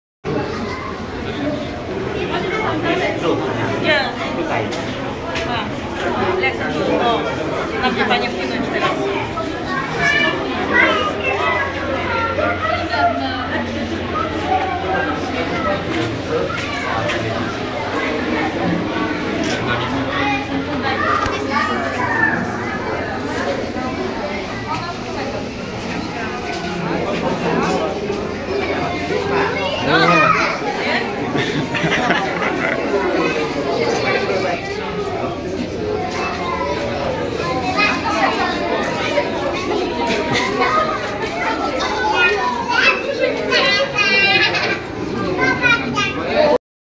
Afrykarium 2015 : Płaszczka nad zwiedzającymi
Tunel który umożliwia obserwację zwierząt z różnych stron